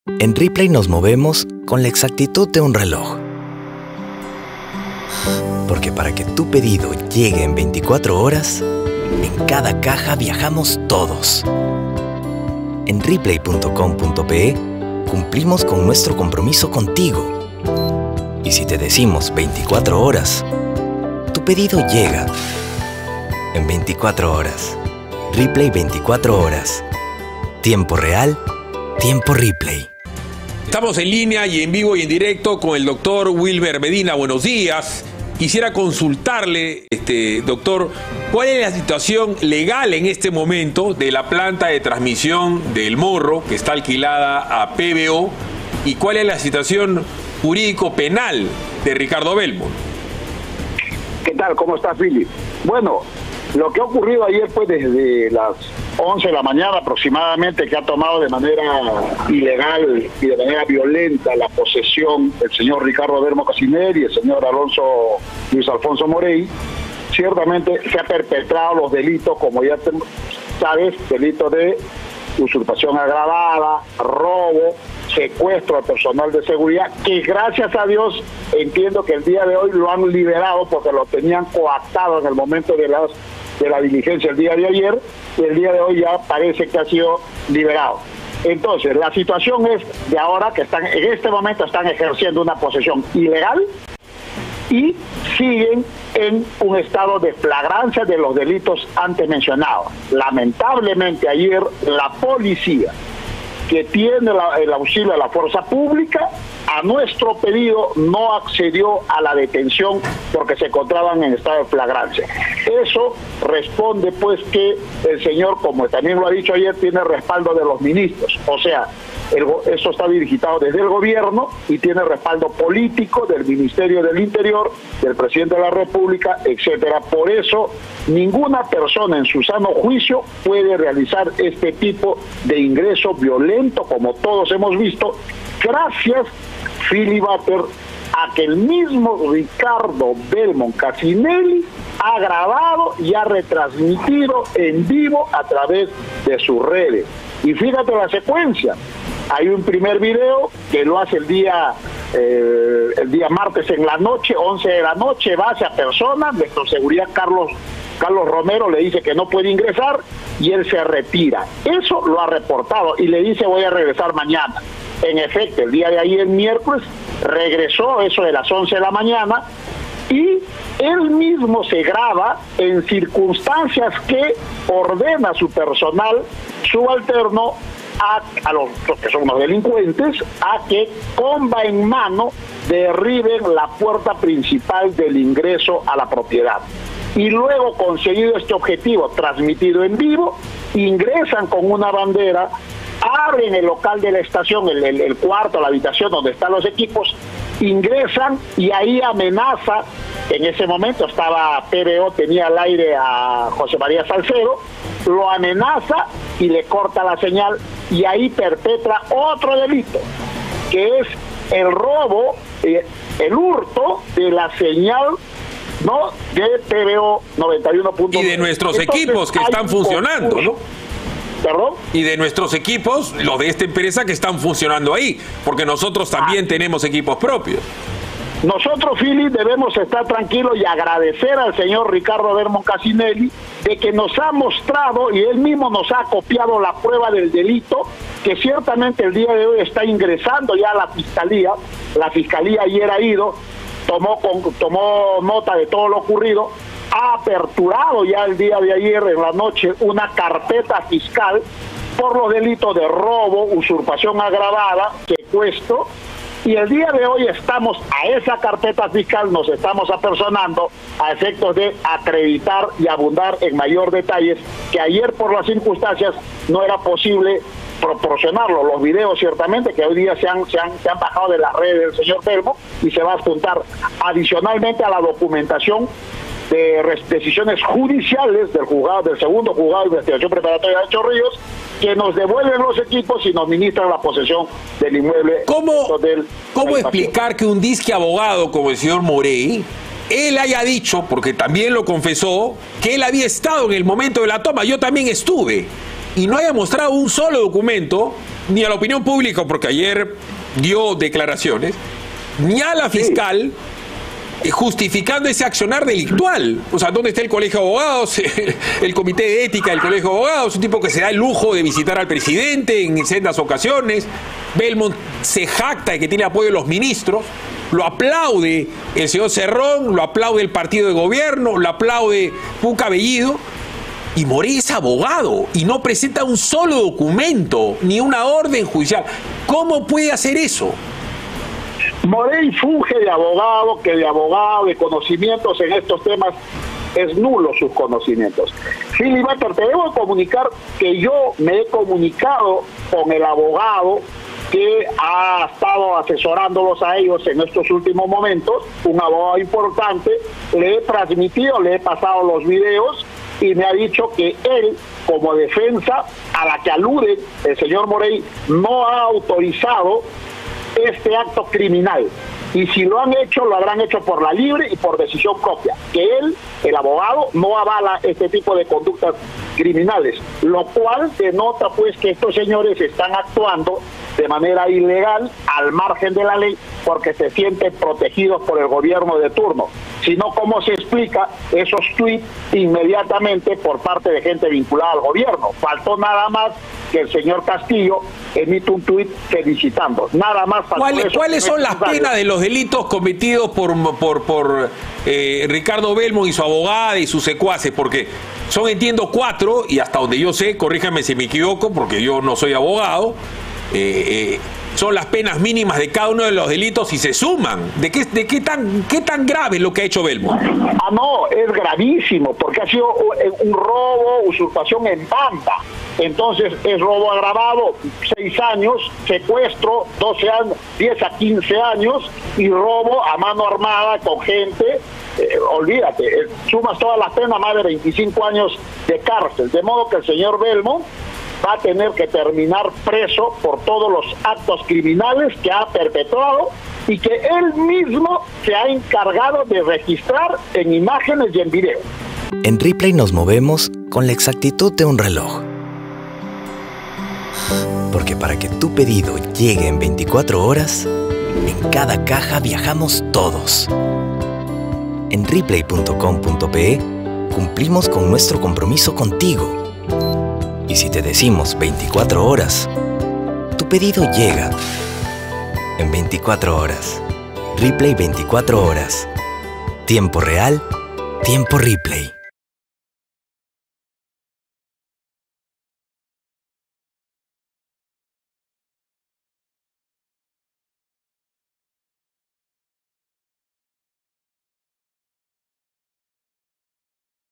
Abogado Wilber Medina sobre Belmont: "va a tener que terminar preso"
PBO | Abogado Wilber Medina: "El señor Belmont va a tener que terminar preso por todos los actos criminales que ha perpetrado"